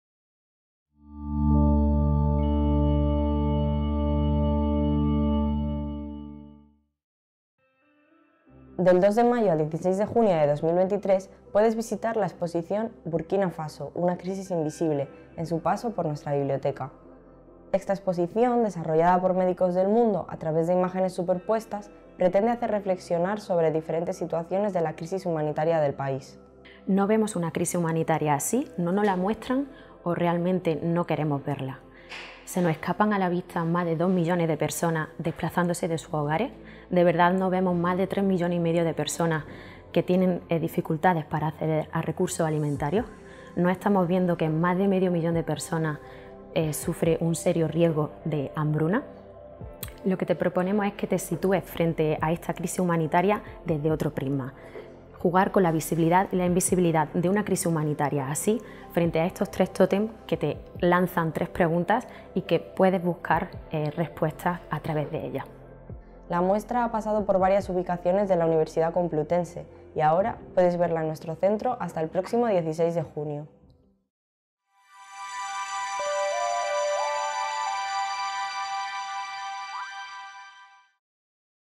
Exposición “Burkina Faso: una crisis (in)visible”
Hasta el próximo 16 de junio se aloja en la sala de lectura de la biblioteca la exposición de Médicos del Mundo “Burkina Faso: una crisis (in)visible”, para concienciar de la crisis humanitaria que está viviendo el país.
Alba Villén (Comunicación Médicos del Mundo) nos presenta la exposición,  un concepto visual en el que a través de imágenes superpuestas pretender hacer reflexionar sobre diferentes situaciones de la crisis humanitaria del país y trata de mostrar como a simple vista hay situaciones invisibilizadas, pero que solo hay que indagar un poco para poder ver y ser conscientes de las diferentes caras de una realidad.
La muestra ha pasado por varias ubicaciones de la Universidad Complutense y ahora puedes verla en la Biblioteca de la Facultad de Enfermería, Fisioterapia y Podología hasta el próximo 16 de junio.